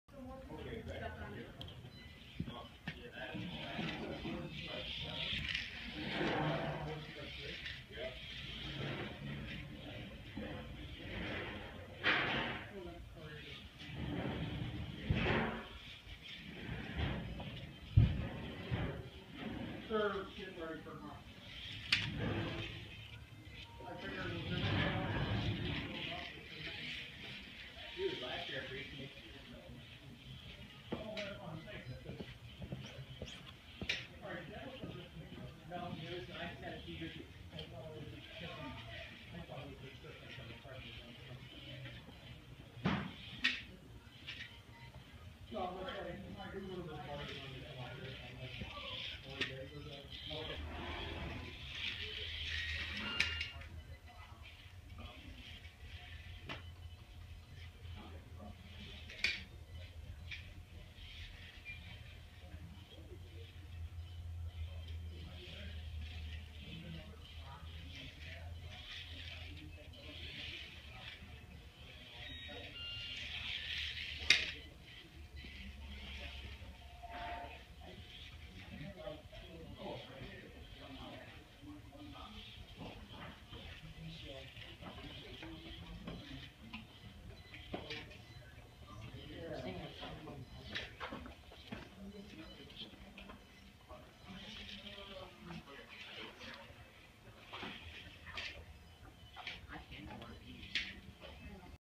6amRC. Hpi Q32 buggy on MiniZ track. HobbyTown Orland Park
First real drive in a track with the Q32. I need lots of practice.